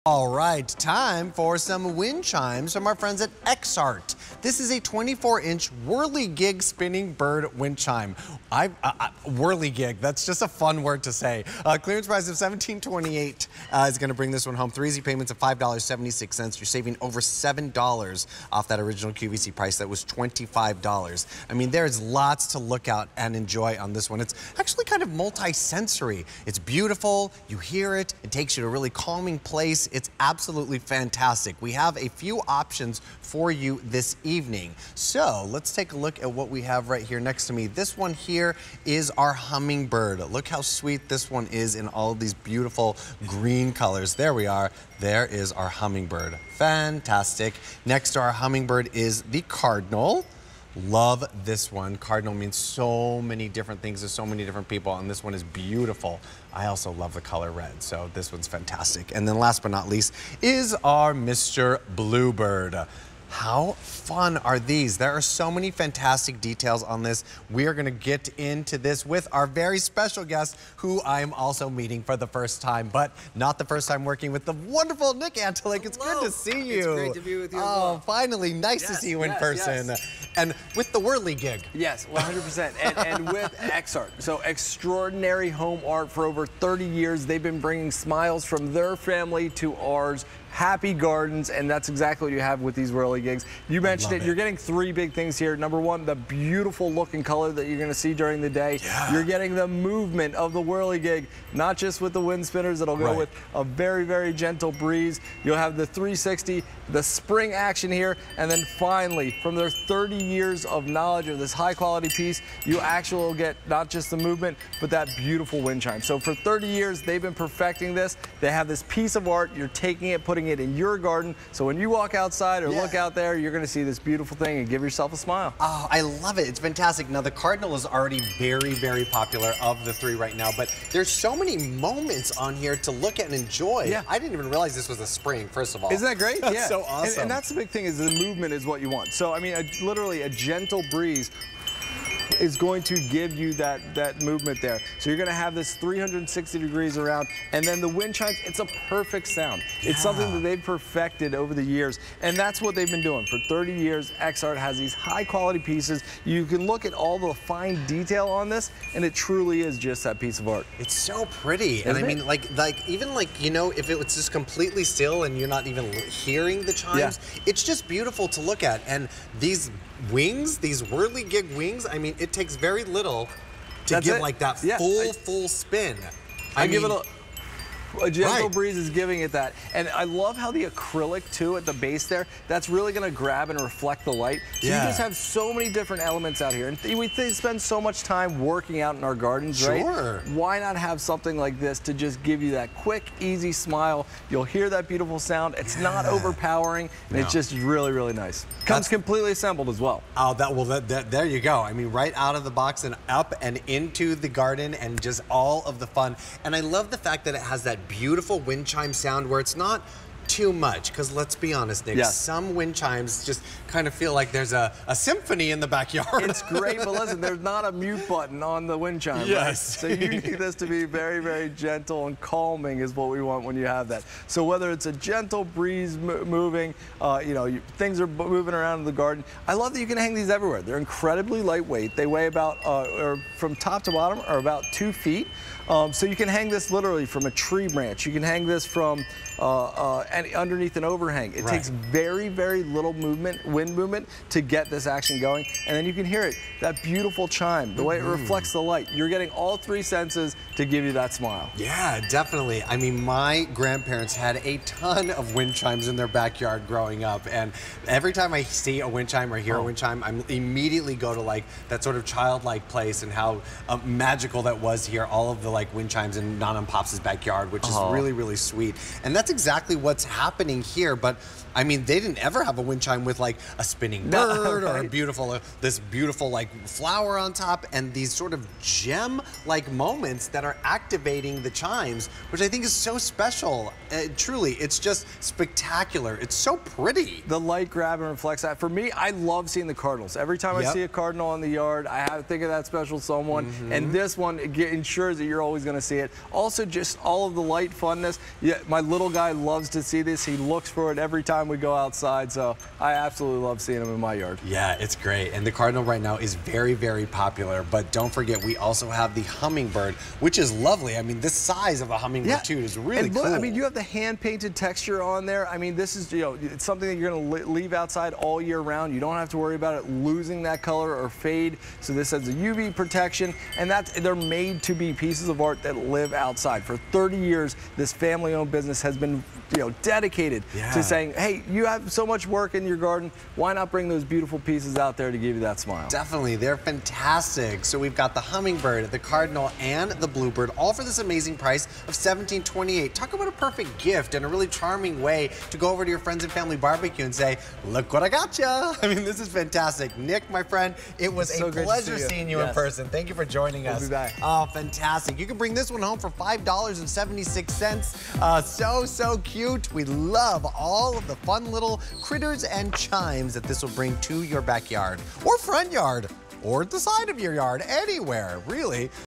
Exhart 24" Whirligig Spinning Bird Windchime Spinner Wings on QVC
Exhart 24" Whirligig Spinning Bird Windchime Spinner Wings

Just a gentle breeze makes this whirligig  bird come alive with spinning motion and the relaxing sounds of chimes. A great gift for the bird lover or colorful focal point for your garden. From Exhart.